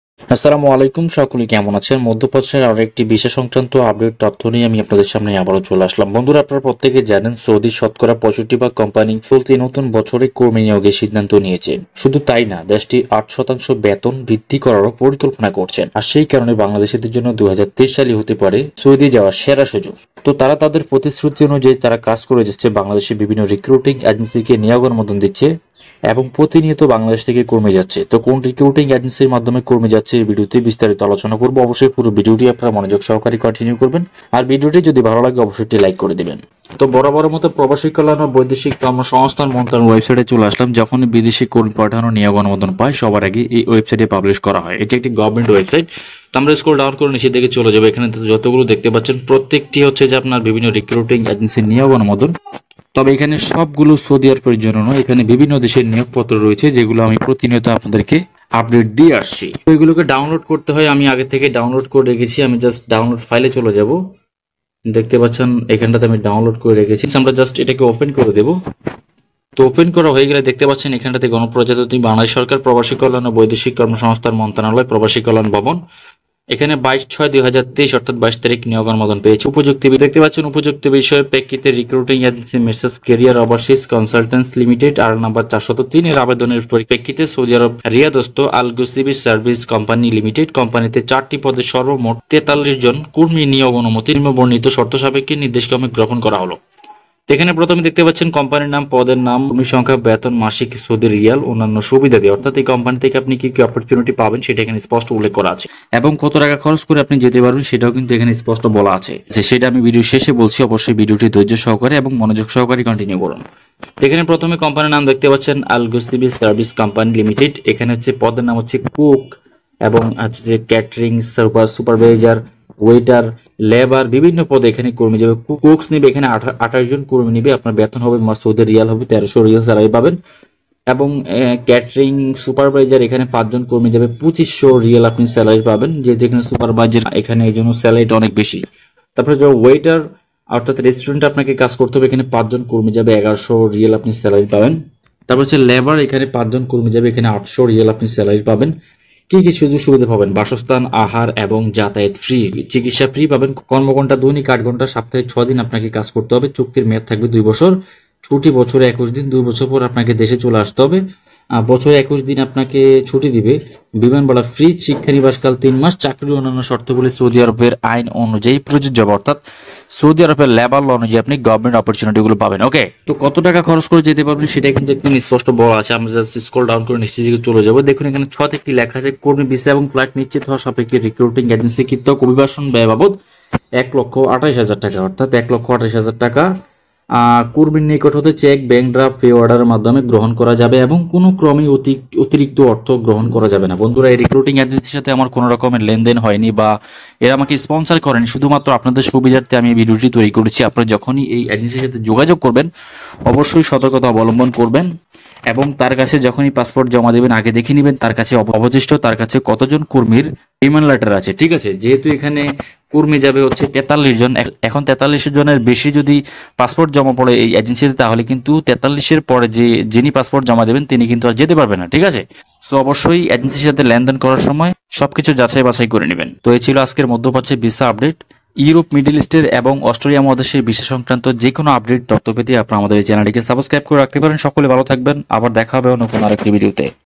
সৌদি বিখ্যাত কোম্পানি কর্মী নিচ্ছে||Saudi Arab work permit VISA 2023
Saudi arabia work visa at nominal cost than saudi arabia worker, saudi arabia cleaning visa taking female workers and male workers see which agencies are sending workers to saudi arabia.

সৌদির শতকরা ৬৫ ভাগ কোম্পানি চলতি নতুন বছরে কর্মী নিয়োগের সিদ্ধান্ত নিয়েছে। শুধু তাই না, দেশটি ৮ শতাংশ বেতন বাড়ানোরও পরিকল্পনা করছে। আর সে কারণে বাংলাদেশীদের জন্য ২০২৩ সালই হতে পারে সৌদি যাওয়ার সেরা সুযোগ।